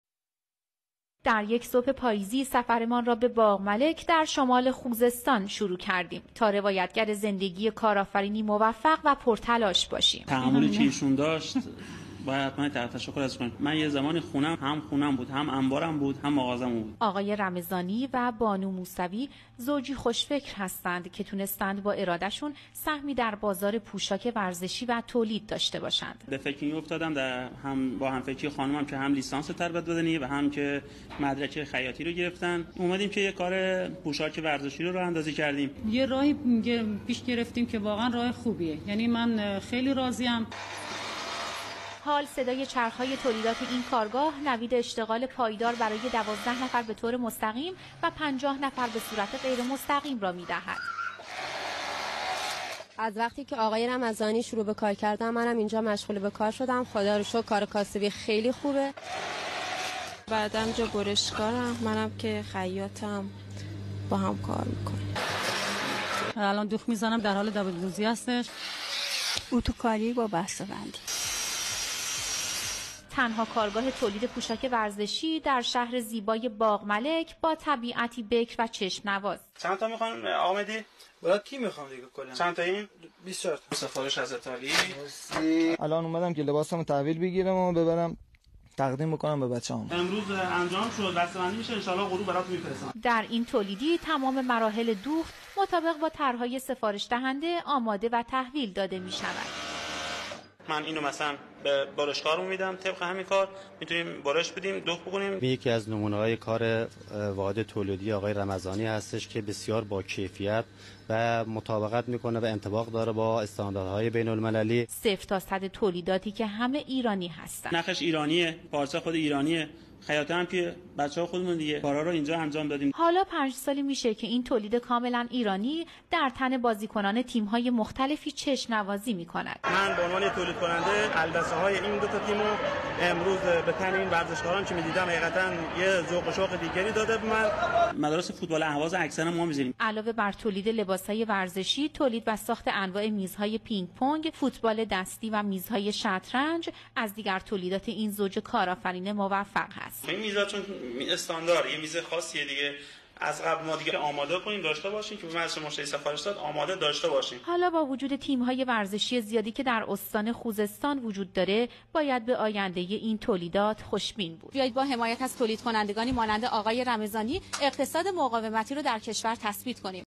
Iran made Sport wears manufacturer, Bagh-Malek city توليد پوشاك ورزشي شهر باغ ملك ايران
Iran made Sport wears manufacturer, Bagh-Malek city
توليد پوشاك ورزشي شهر باغ ملك ايران